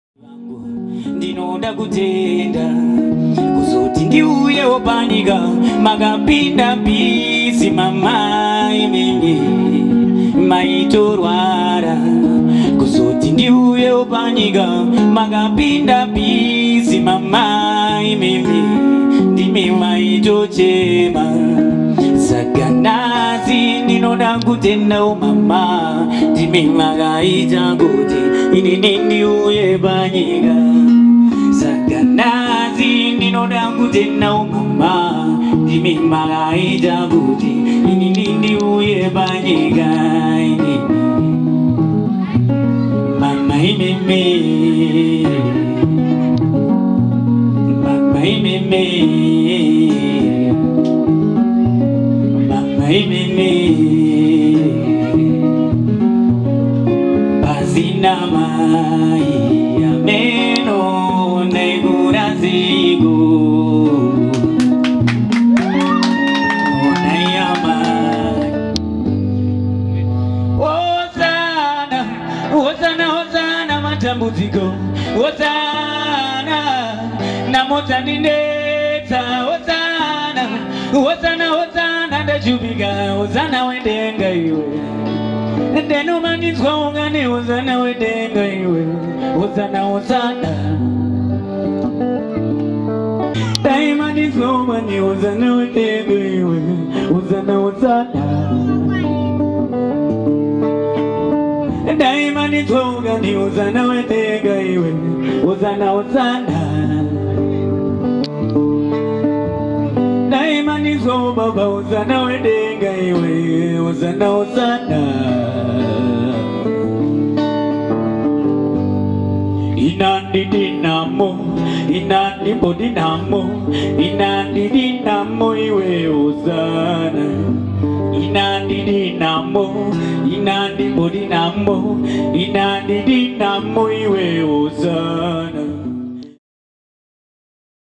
Jay Watta performing at a charity event in Norton/Zimbabwe
Solo Performance